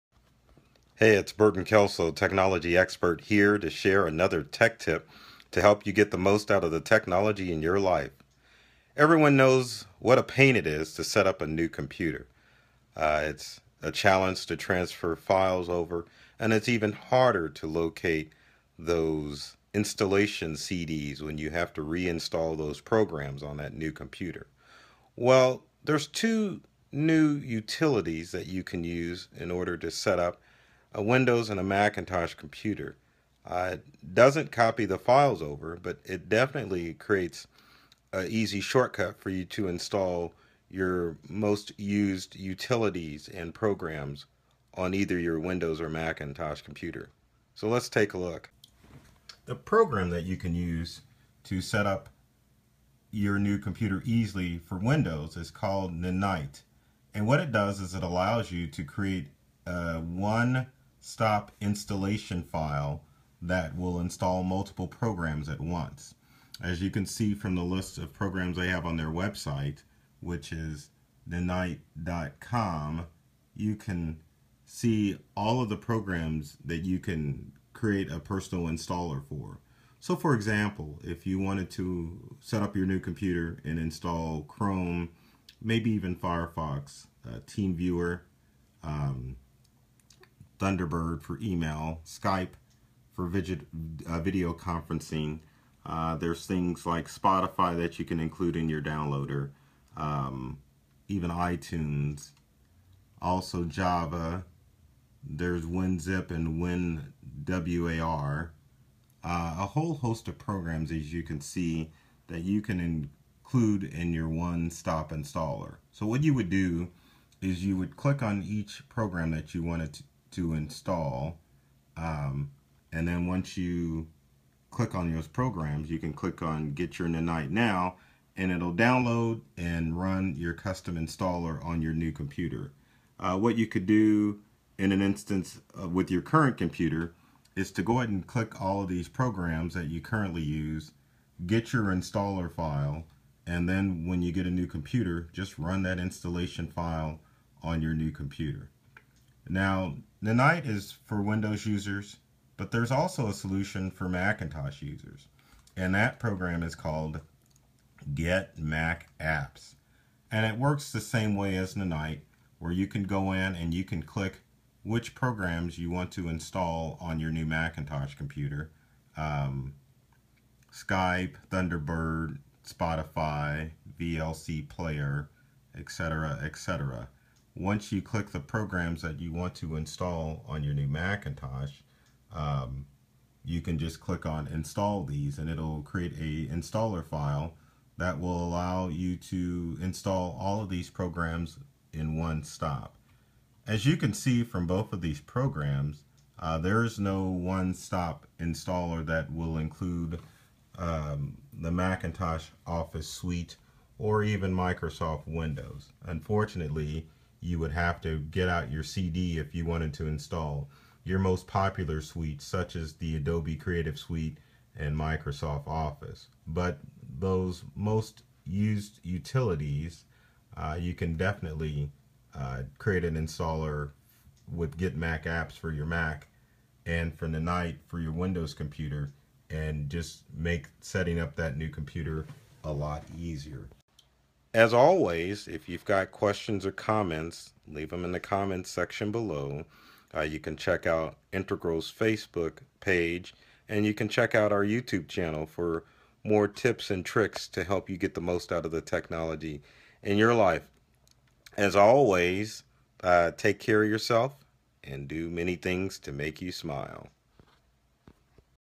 Tech Tips for Non Tech People | Setup that New Computer in a Snap
Watch this video to learn how to setup your new computer quickly. Have questions, post them in the comments section.